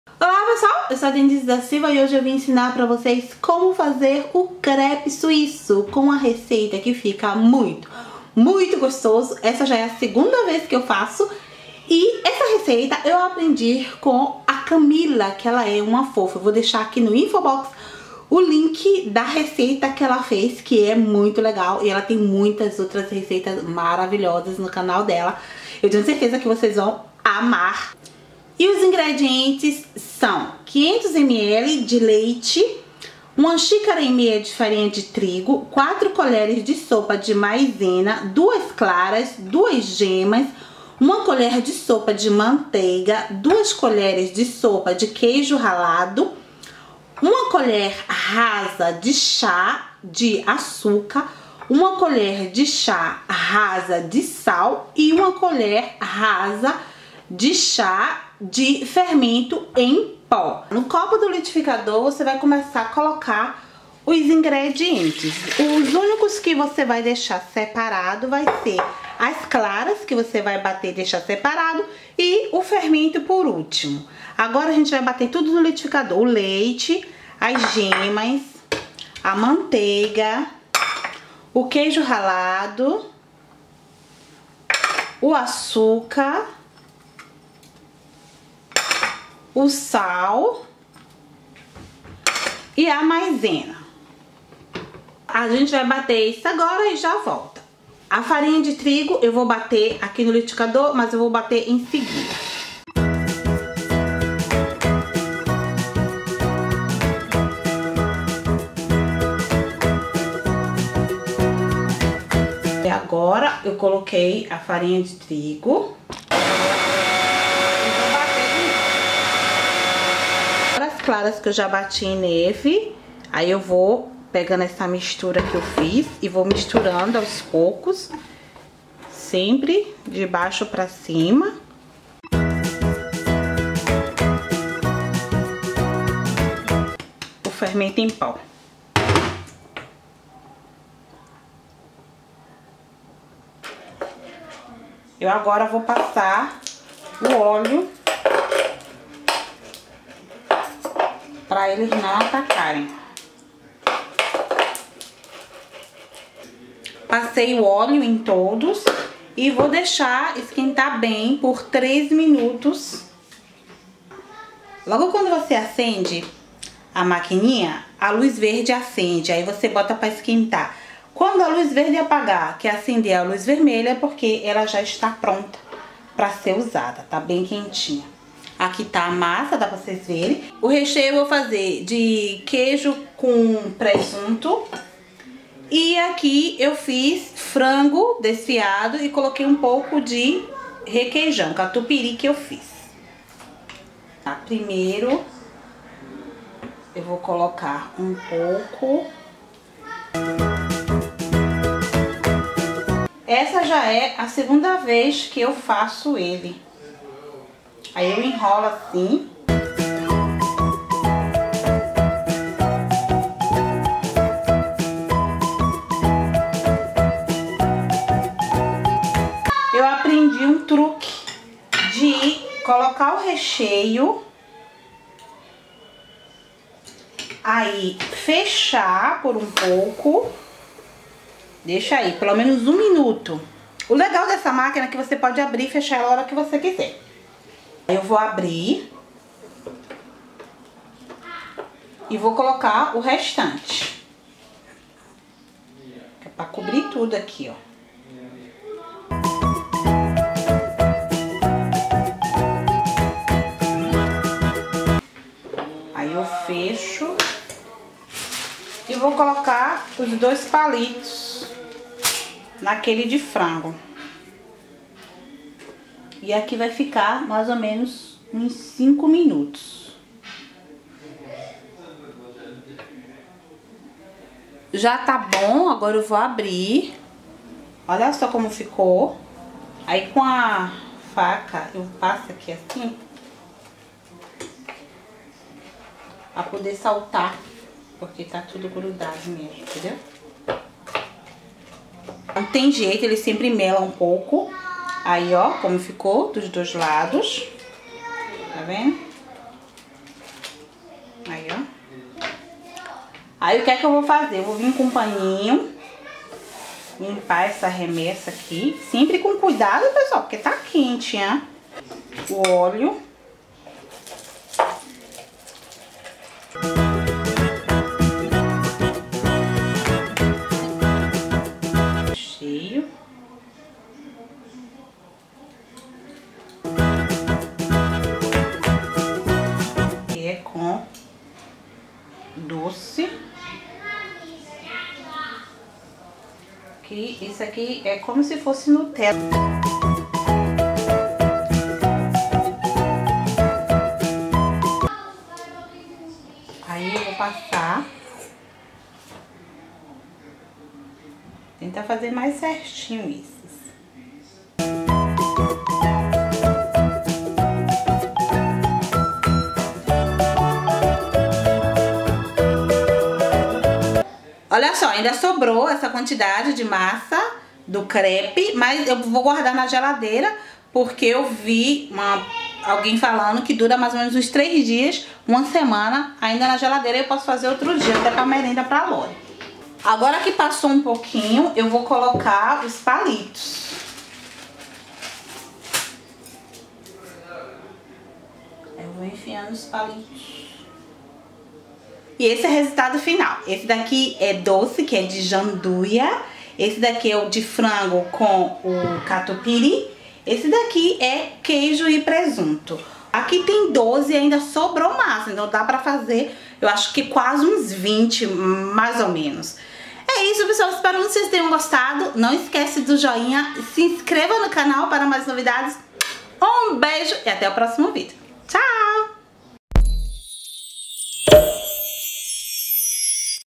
Crepe Suíço Rápido e Fácil |Aldenildes Silva
Oi Pessoal hoje vim fazer para vocês essa receita de Crepe Suíço Rápido e Fácil, já fiz ela e amei, uma uma delicia o crepe, crocante fora e mórbido por dentro, Espero que Gostem!
E fiquem ligadas nos próximos videos, Mil Beijos!

❤ SNAPCHAT: @buonovivere - Aldenildes Silva

★ Assista  Também ★